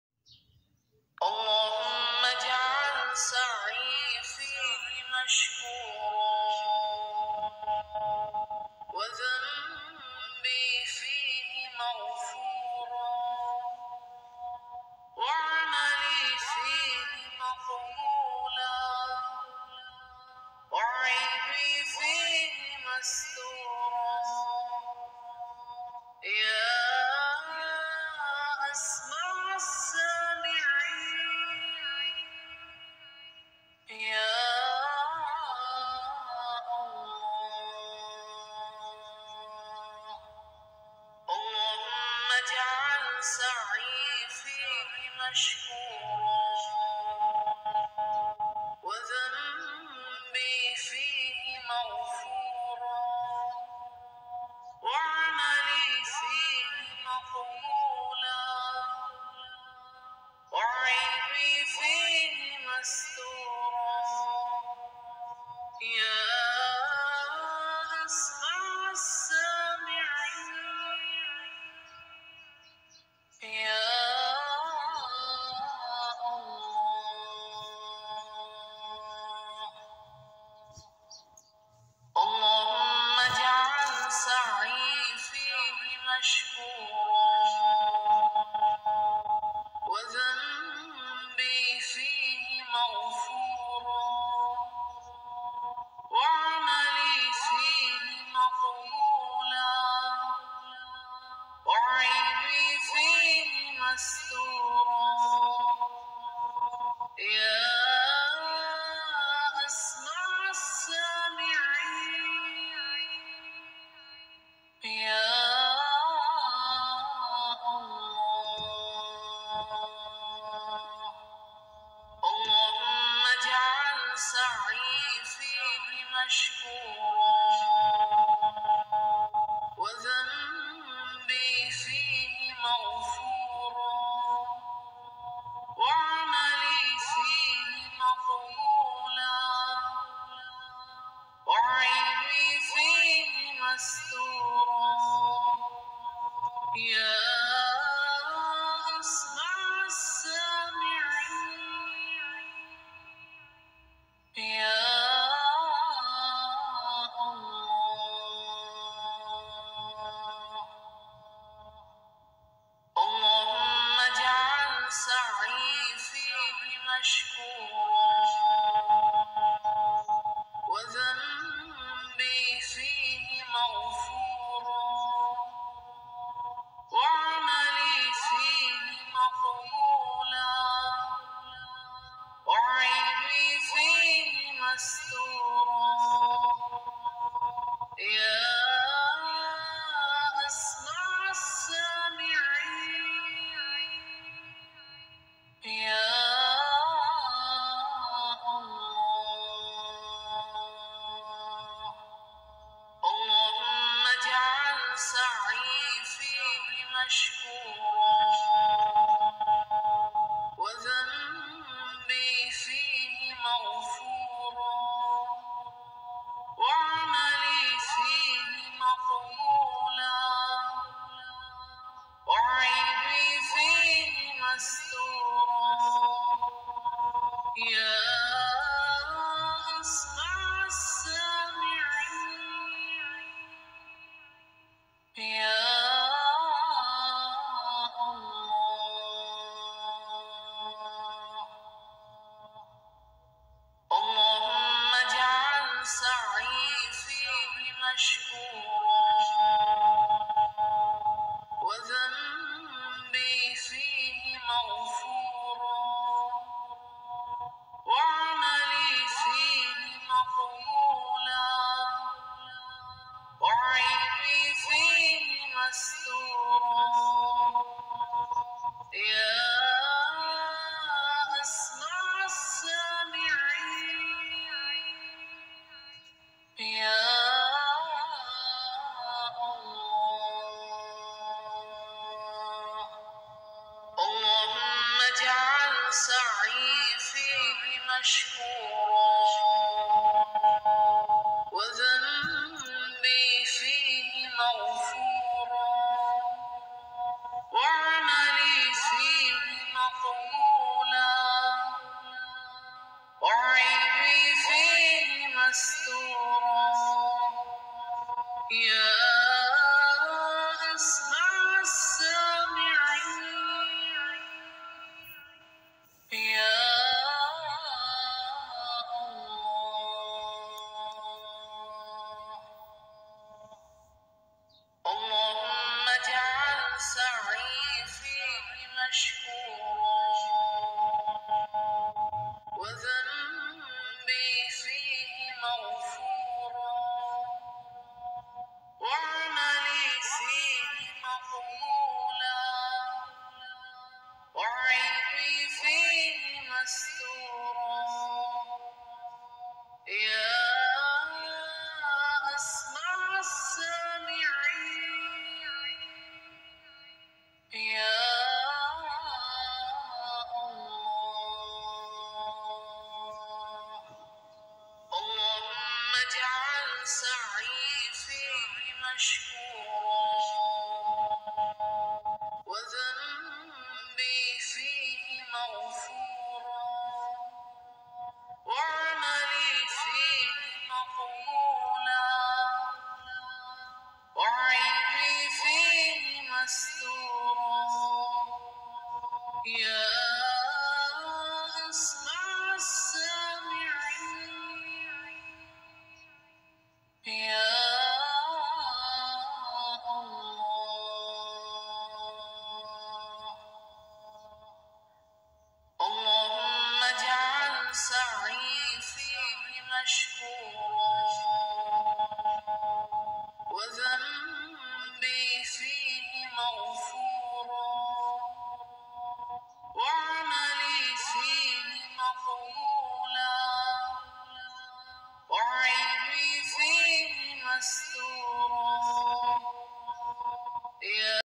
Listen Daily This Dua In Ramadan 2024-Must Listen
Listen Daily This Dua HD n Ramadan 2024

Must Listen!!!

Dua Of The Day [HD]  - A very beautiful Dua Must Watch

Dua Of The Day!!

"O Allah, make my efforts worthy of appreciation  , and my sins forgiven , my deeds accepted, my flaws concealed , O The Best of those who hear.
Ameen

You will be rewarded for All our future Quran Recitation & Supplication , In Sha Allah
JazzaAllahu Khairan for your Support